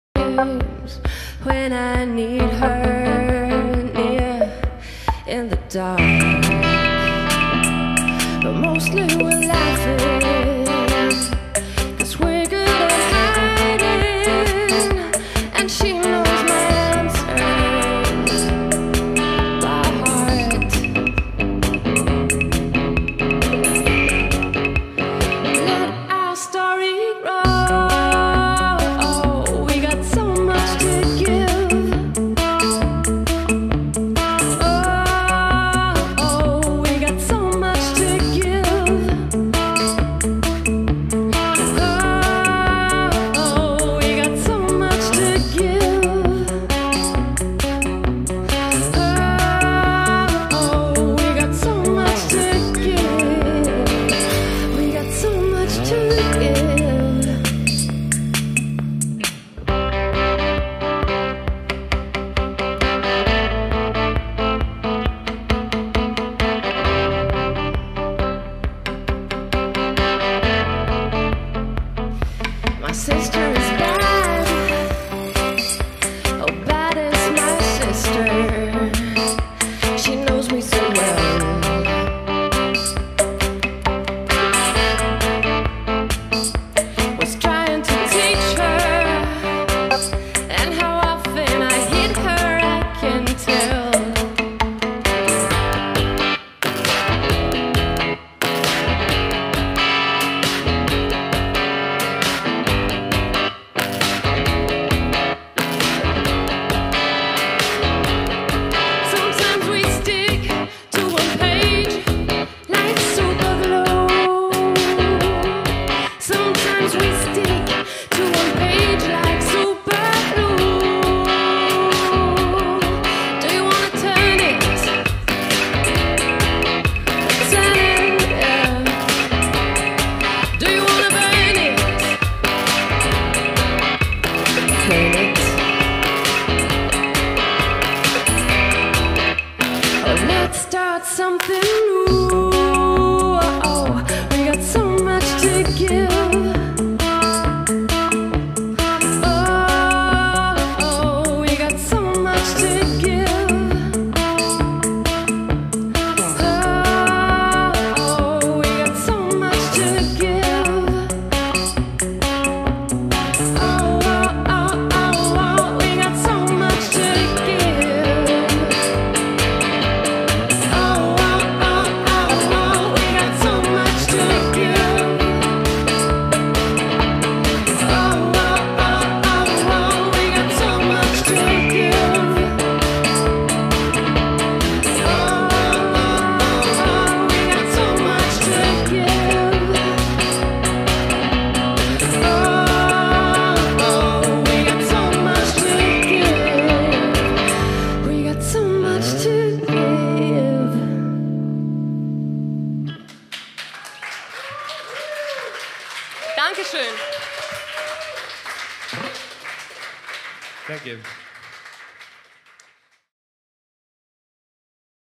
Sad Sister, LA FONS live in der Metastadt
Sad Sister, LA FONS live in der Metastadt, Wien 22, bei der Eröffnung des Kulturfestivals Donaustadt Auf Zu Neuen Ufern im August 2012. Pan Danubia (Vernissage von Stefan Fürtbauer) und La Fons